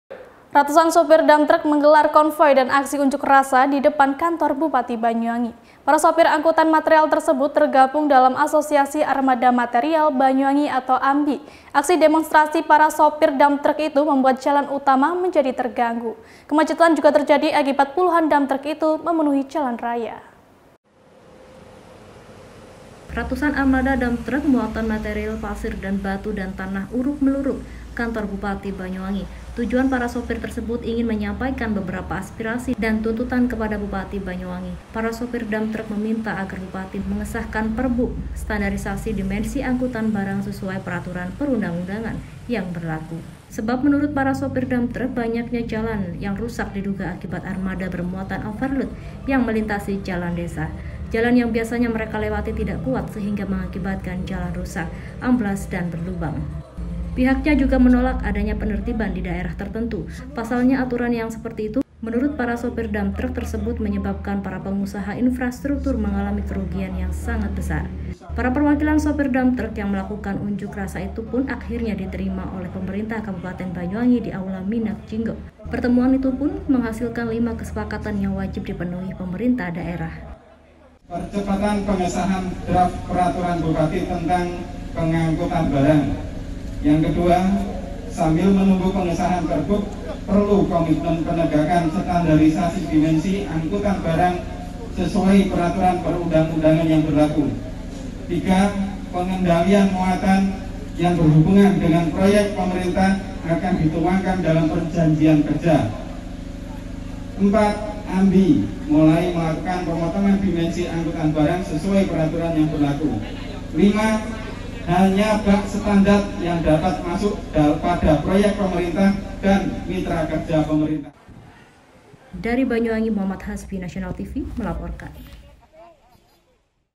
ASOSIASI ARMADA MATERIAL BANYUWANGI (AMBI) GERUDUK KANTOR BUPATI
Banyuwangi, 20 September 2021

Rogojampi - Banyuwangi